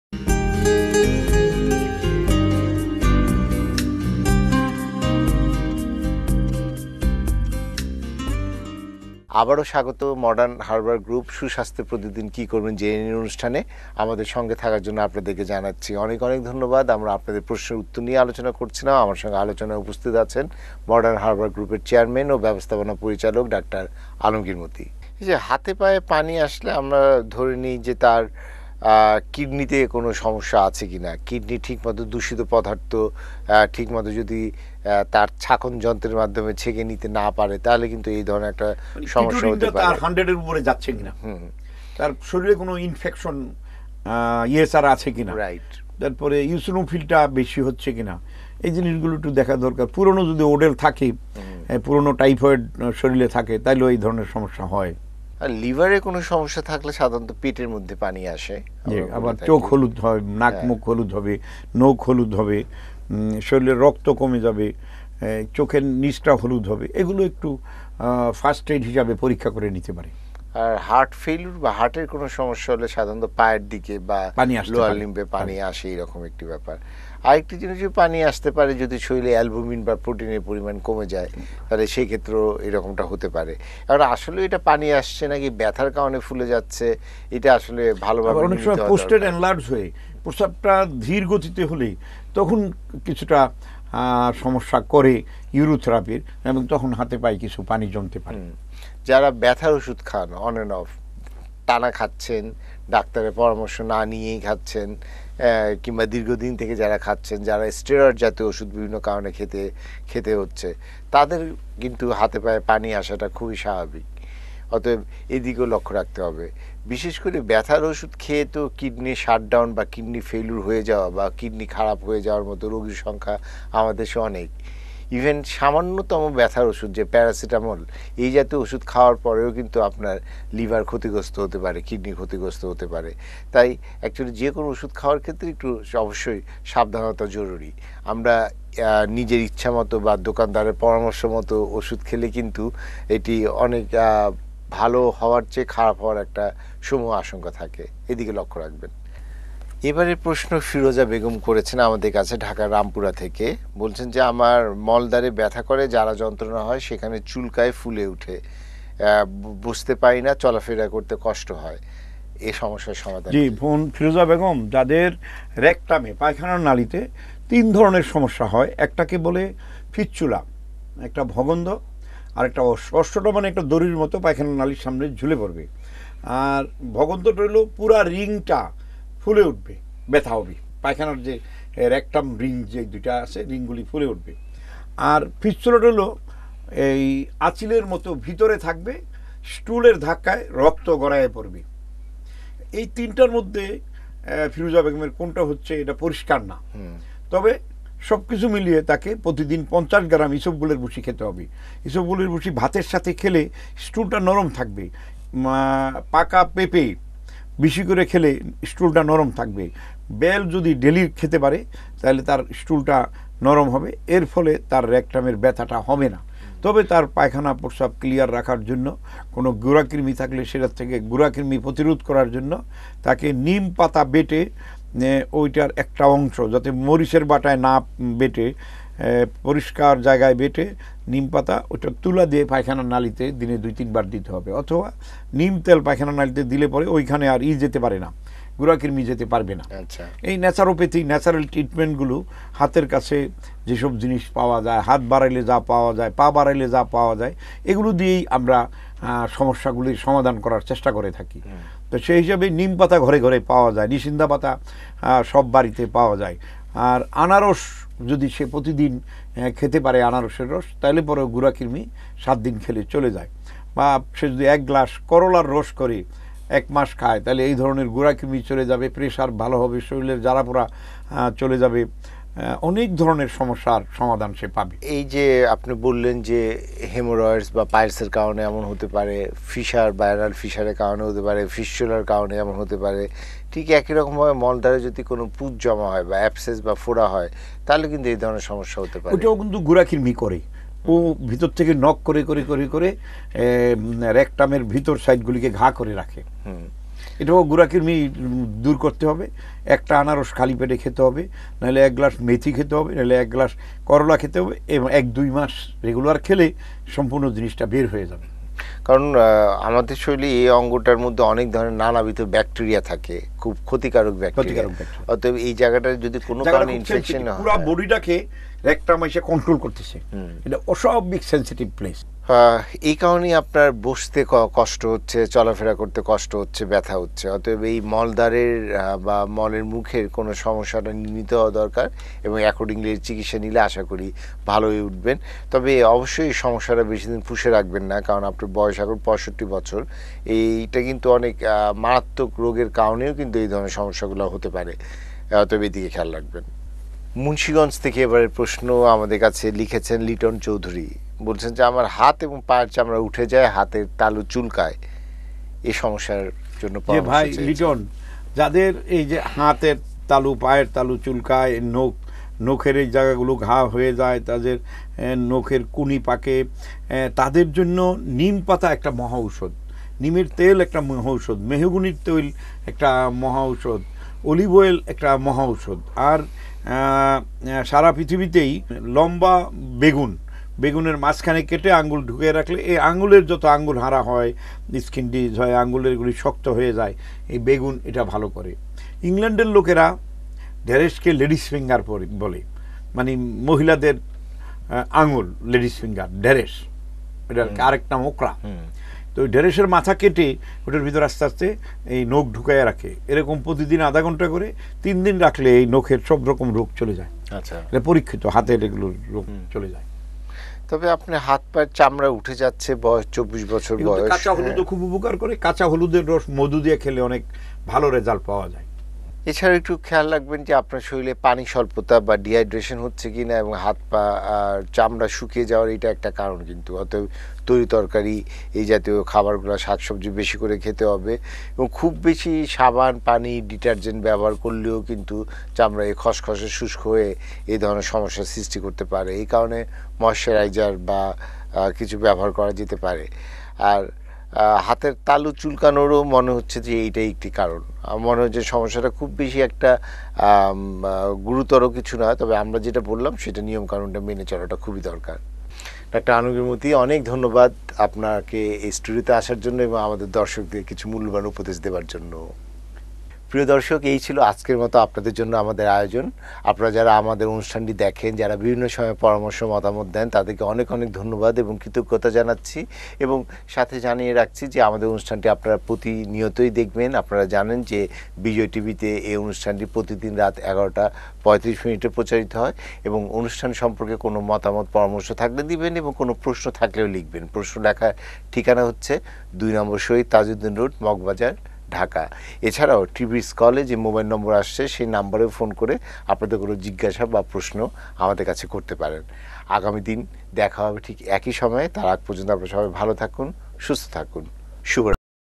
হাতে-পায়ে পানি জমা, মলদ্বারে ব্যথা, চুলকায় ও হাপ পায়ের চামড়া উঠে যায়। Eps 69 Part 02
আলোচনা করছেন- বিশিষ্ট হারবাল গবেষক ও চিকিৎসক
ডা. আলমগীর মতি
চেয়ারম্যান ও ব্যবস্থাপনা পরিচালক
মডার্ণ হারবাল গ্রুপ
ঢাকা, বাংলােদশ।
ফোন :  ০১৯১৩-৫৬০০৯৫